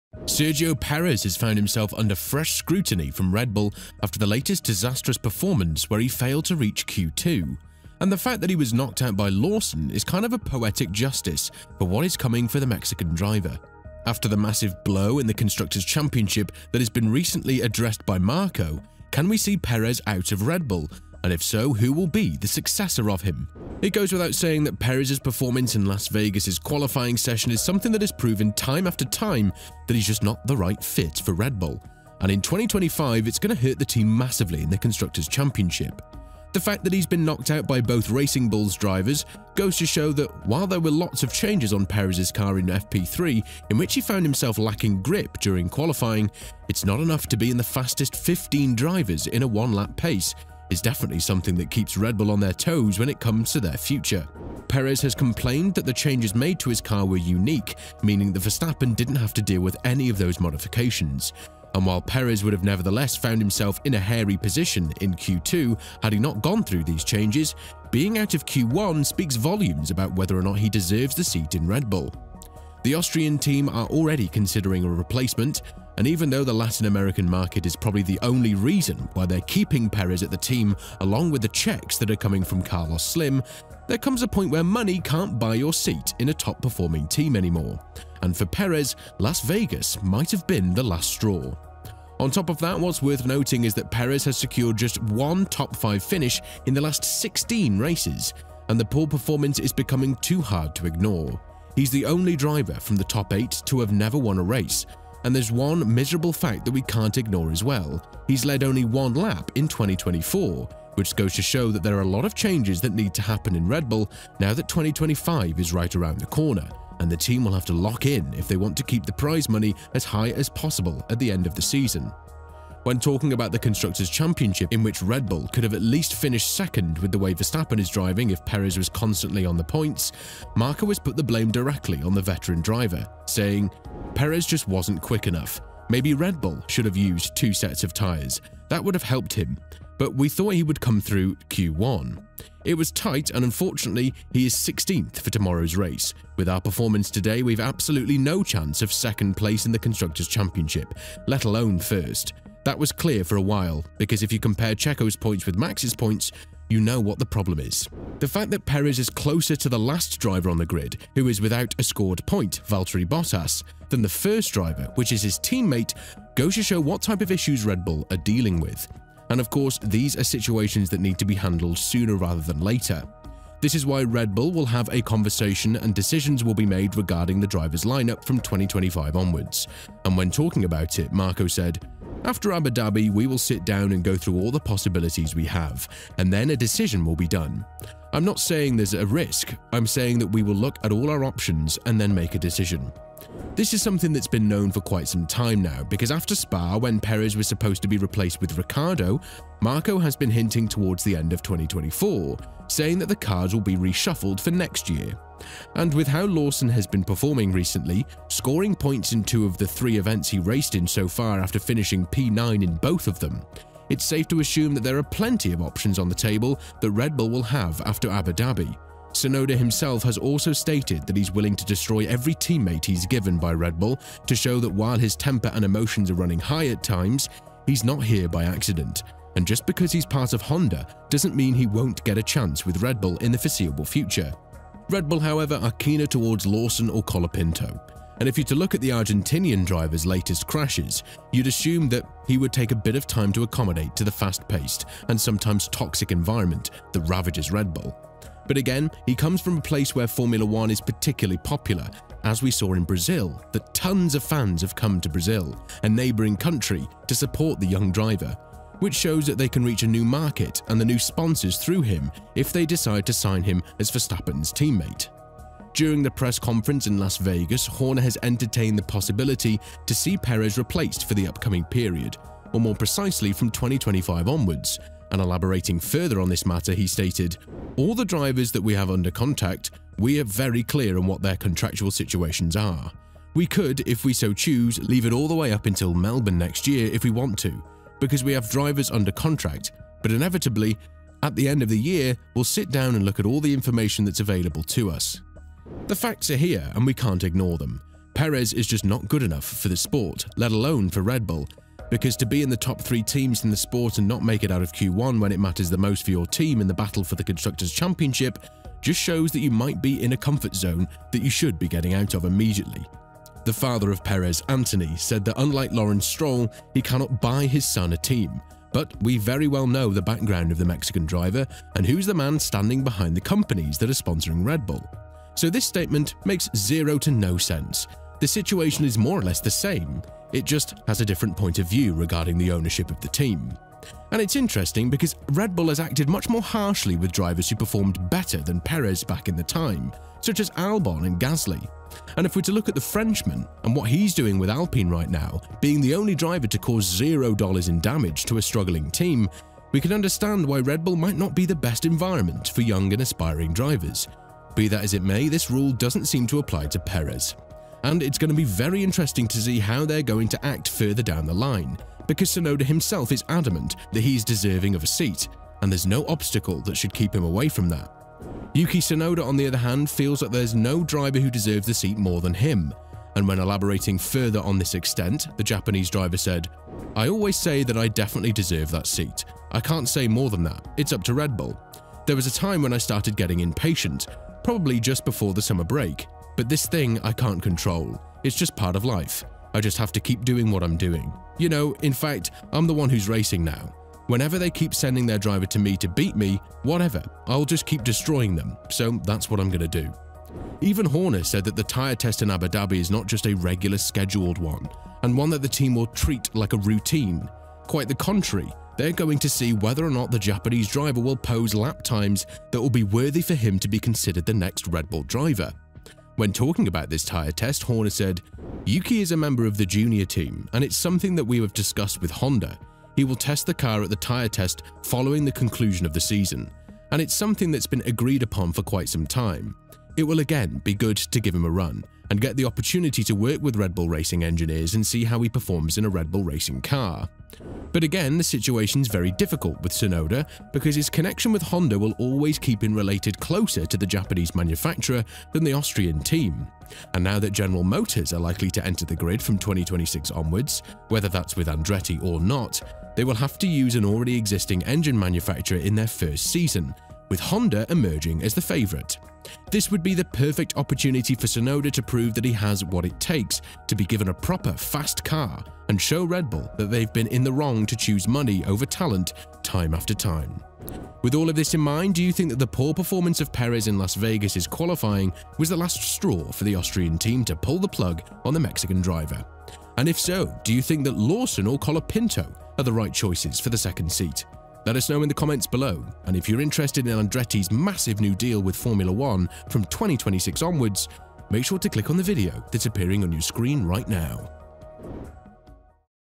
Red Bull READY for Tsunoda to REPLACE Perez After SHOCKING Las Vegas FAILURE!
■ Sergio Perez has found himself under fresh scrutiny from Red Bull after the latest disastrous performance where he failed to reach Q2, and the fact that he was knocked out by Lawson is kind of a poetic justice for what is coming for the Mexican driver. After the massive blow in the constructors’ championship that has been recently addressed by Marko, can we see Perez out of Red Bull, and if so, who will be the successor of him?